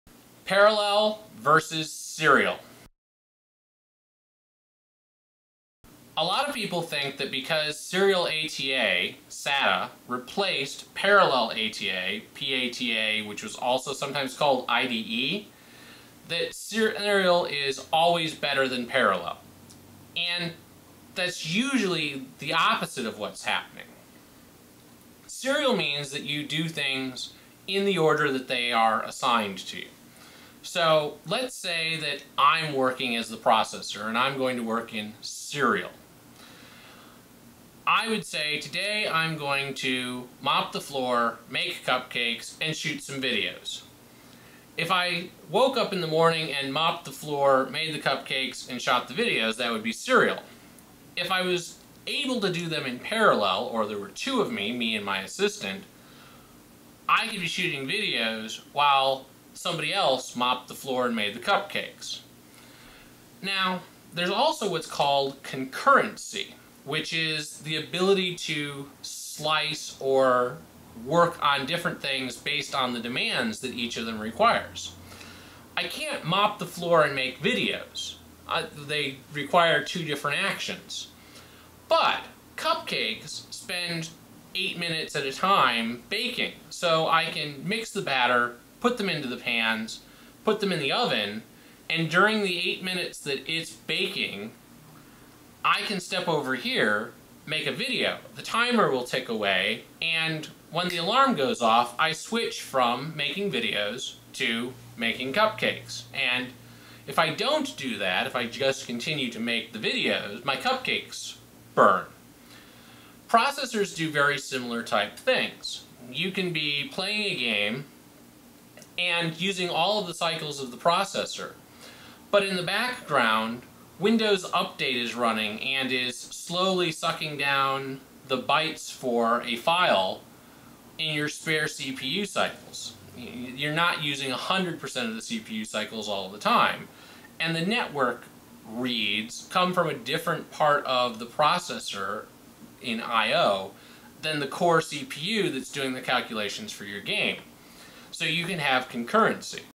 Parallel Vs. Serial
There are some misconceptions that Serial is always better than Parallel. In fact usually the opposite is true. So I give some examples of each.